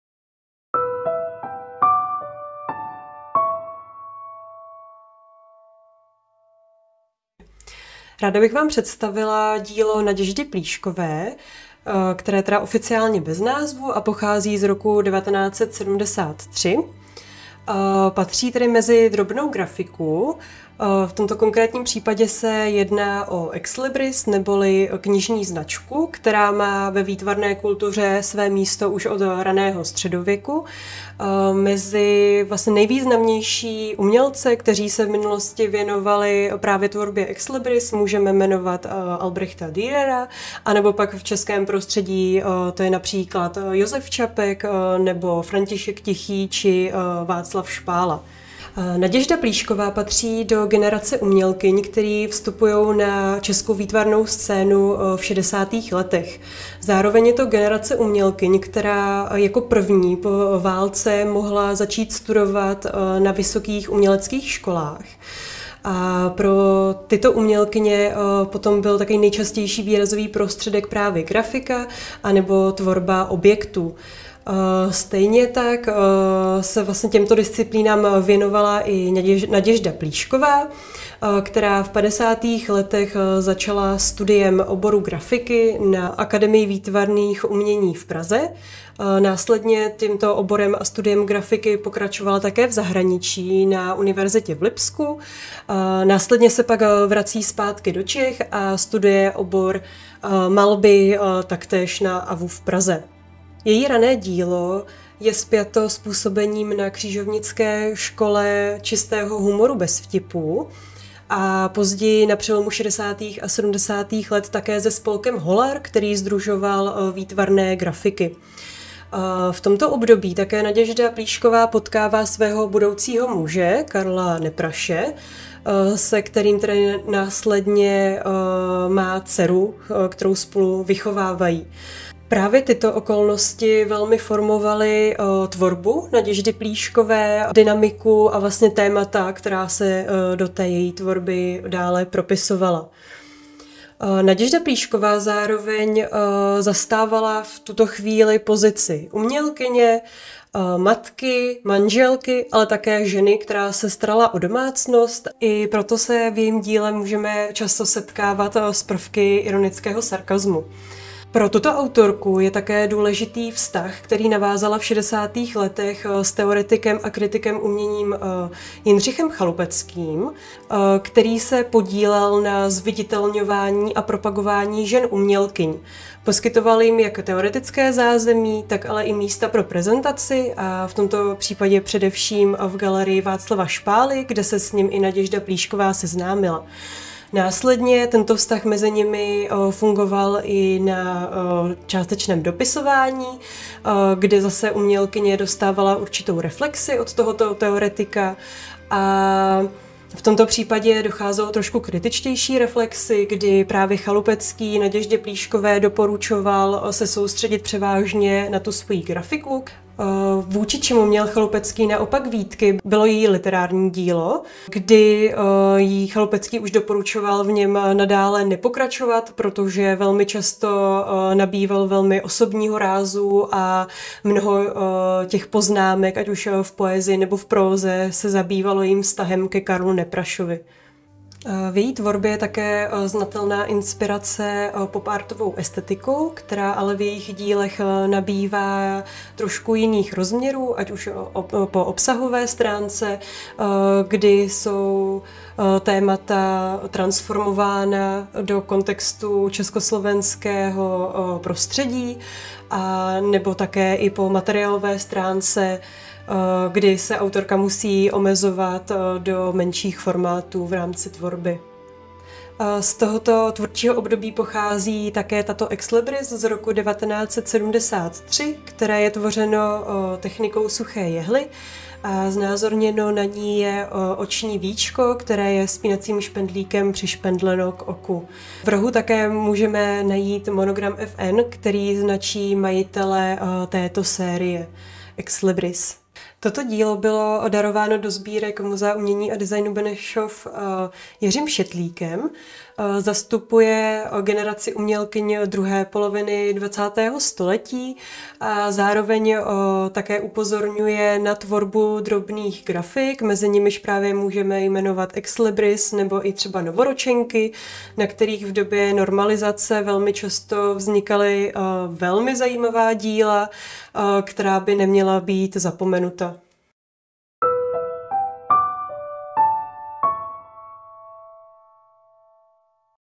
bez názvu, 1973 (Naděžda Plíšková)
Grafika bez názvu datovaná rokem 1973, na které je zobrazeno ženské oční víčko probodnuté spínacím špendlíkem, je autorským dílem grafičky a básnířky Naděždy Plíškové, členky umělecko-recesistického společenství Křižovnická škola čistého humoru bez vtipu založené na konci 50. let 20. století. V dobových souvislostech je její dílo výjimečné reflexí společenského kontextu v podobě genderové kritiky a kritiky konzumní společnosti. Tuto problematiku Plíšková ironicky demonstrovala v souvislosti s vlastním postavením umělkyně, matky, manželky a ženy starající se o domácnost. Jejím tradičním výrazovým prostředkem se jí stala právě grafika a později v období normalizace, kdy jí bylo zakázáno tvořit, i poezie. Podporu své tvorby našla v teoretikovi Jindřichovi Chalupeckém, který označoval její vnímání reality jako lyrický sarkasmus. Pro její tvorbu je příznačná symbolika a rafinovanost.